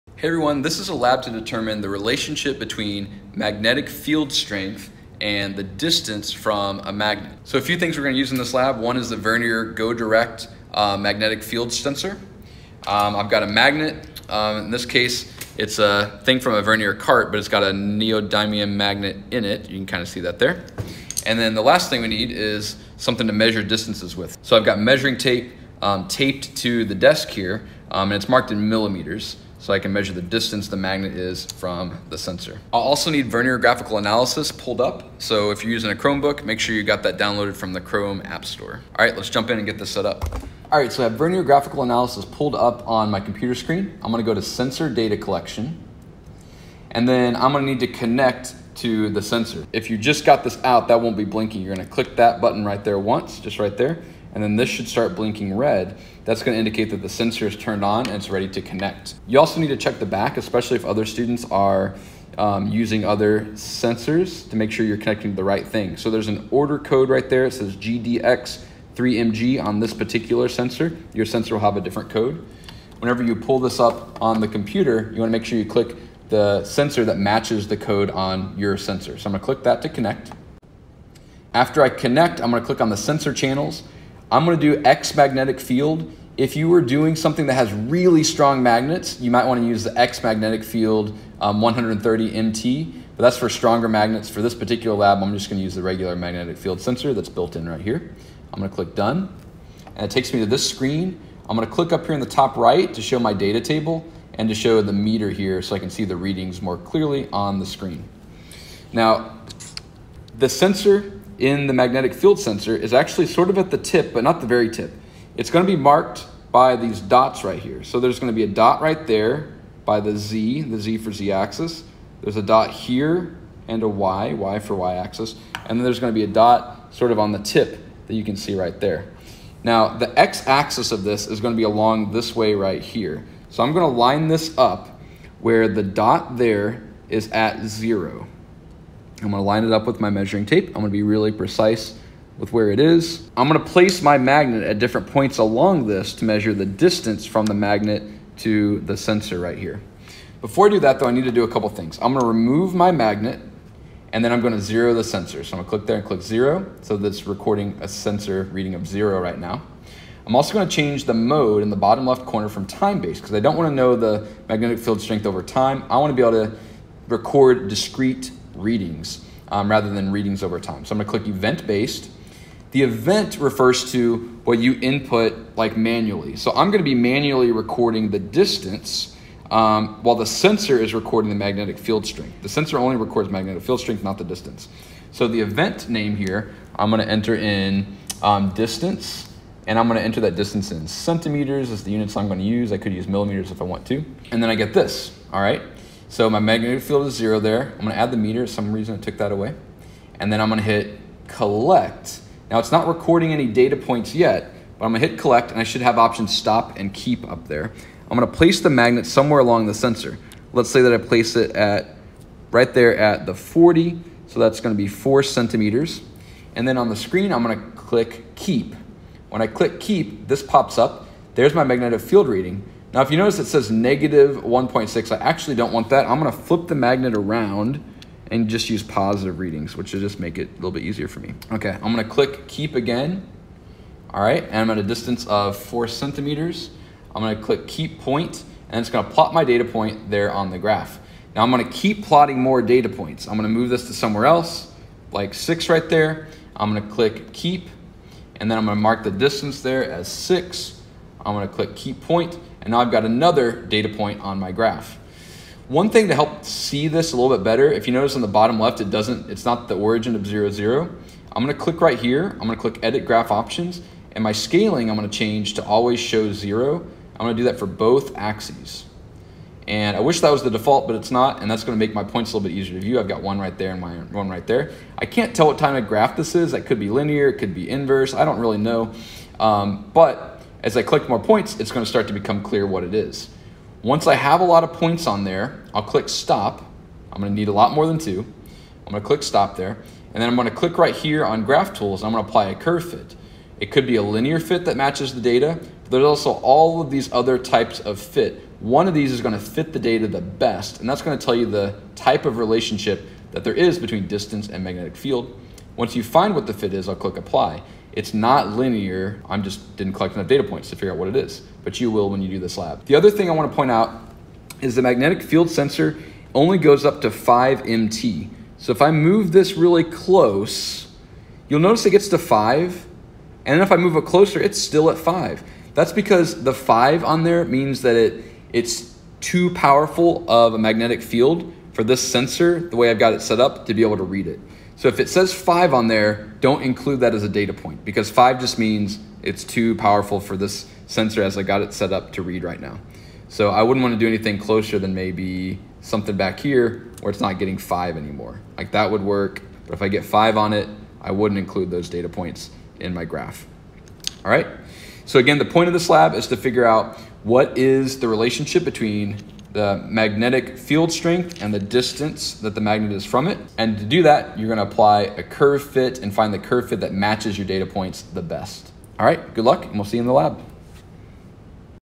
Setup for Magnetic Field Strength vs. Distance Lab using Vernier Sensor
In this video, I show the setup for a lab experiment to determine the relationship between magnetic field strength and distance from the magnet. I use the Vernier Go Direct 3-Axis Magnetic Field Sensor.